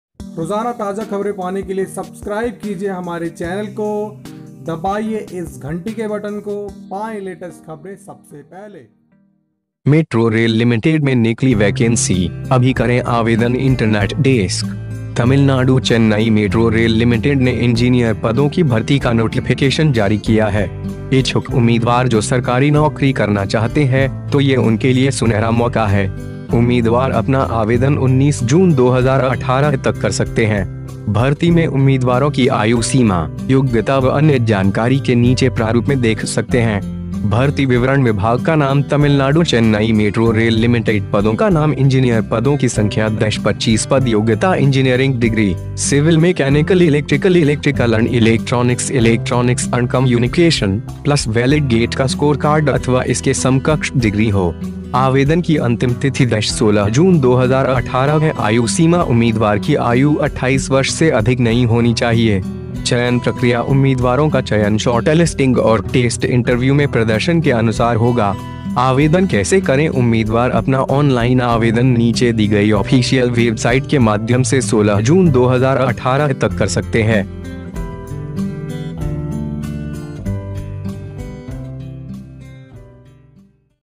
मेट्रो रेल लिमिटेड में निकली वैकेंसी, अभी करें आवेदन
इंटरनेट डेस्‍क। तमिलनाडु चेन्नई मेट्रो रेल लिमिटेड ने इंजीनियर पदों की भर्ती का नोटिफिकेशन जारी किया है। इच्छुक उम्मीदवार जो सरकारी नौकरी करना चाहते है, तो ये उनके लिए सुनहरा मौका है। उम्मीदवार अपना आवेदन 19 जून 2018 तक कर सकते हैं। भर्ती में उम्मीदवारों की आयु सीमा, योग्यता व अन्य जानकारी के नीचे प्रारुप में देख सकते हैं।

भर्ती विवरण
विभाग का नाम - तमिलनाडु चेन्नई मेट्रो रेल लिमिटेड
पदों का नाम - इंजीनियर
पदों की संख्या - 25 पद
योग्यता - इंजीनियरिंग डिग्री (सिविल / मैकेनिकल / इलेक्ट्रिकल / इलेक्ट्रिकल एंड इलेक्ट्रॉनिक्स / इलेक्ट्रॉनिक्स एंड कम्युनिकेशन) + वैलिड GATE का स्कोर कार्ड अथवा इसके समकक्ष डिग्री हो।
आवेदन की अंतिम तिथि - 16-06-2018
आयु सीमा - उम्मीदवार की आयु 28 वर्ष से अधिक नहीं होनी चाहिए।
चयन प्रक्रिया – उम्मीदवारों का चयन शॉर्टलिस्टिंग और टेस्ट / इंटरव्यू में प्रदर्शन के अनुसार होगा।

आवेदन कैसे करें - उम्मीदवार अपना ऑनलाइन आवेदन नीचे दी गयी ऑफीशियल वेबसाइट के माध्‍यम से 16 जून 2018 तक कर सकते है।